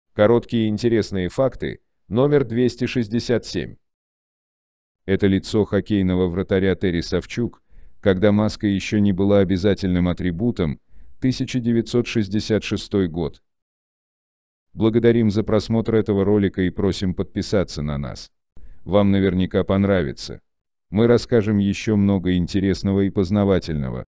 ЛИЦО ХОККЕЙНОГО ВРАТАРЯ ТЕРРИ САВЧУК, КОГДА МАСКА ЕЩЕ НЕ БЫЛА ОБЯЗАТЕЛЬНЫМ АТРИБУТОМ - ФАКТЫ #267
Лицо хоккейного вратаря Терри Савчук, когда маска еще не была обязательным атрибутом.